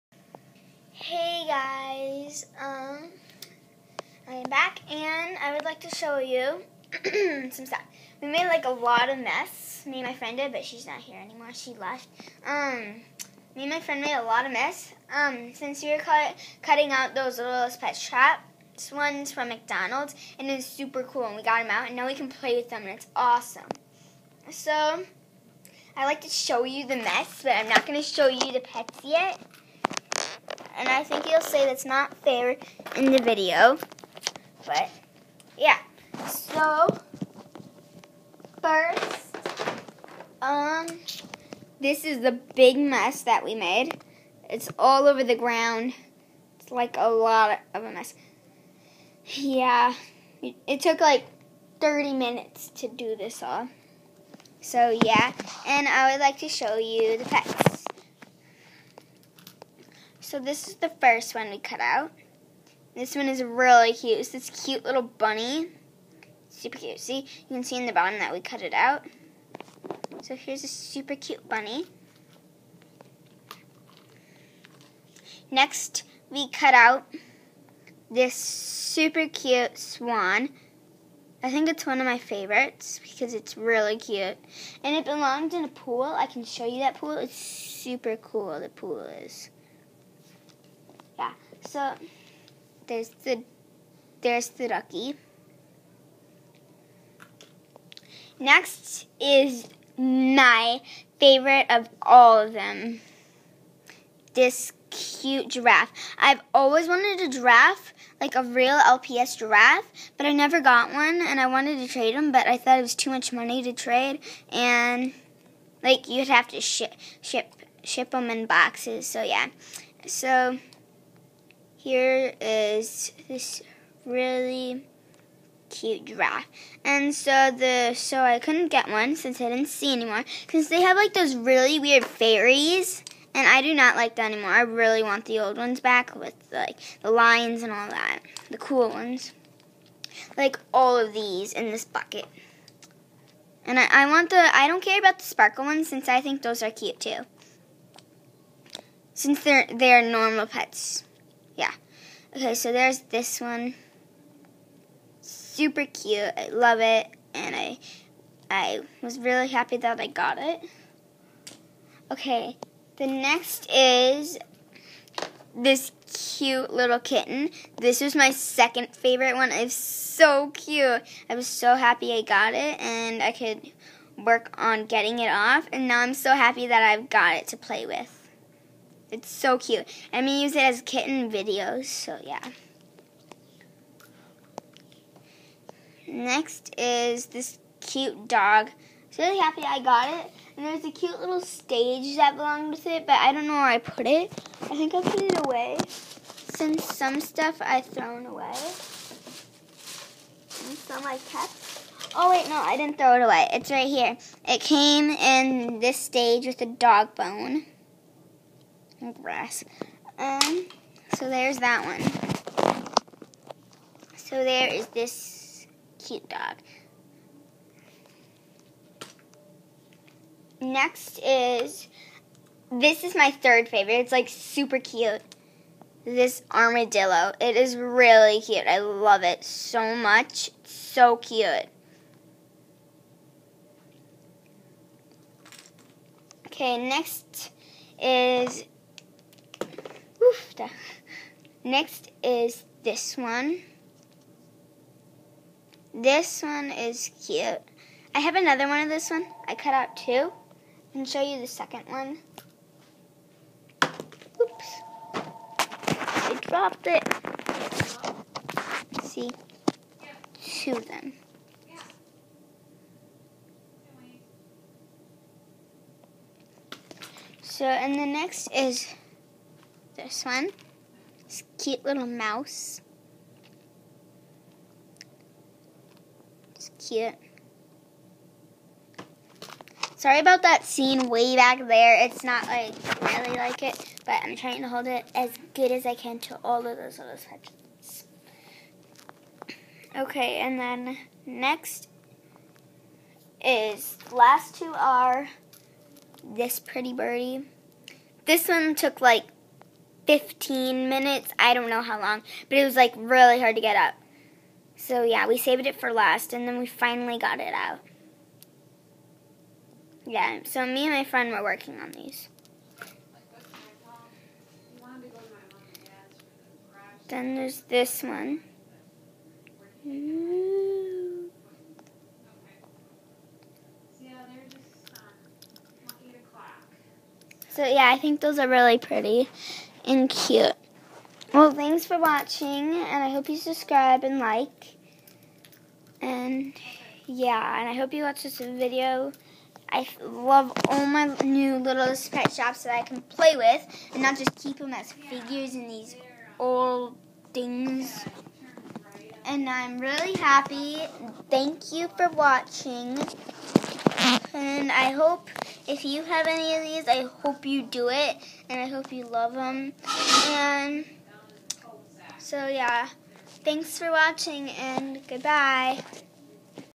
Lps McDonald toy
I am so happy that I got all  these McDonald lps out of there case took forever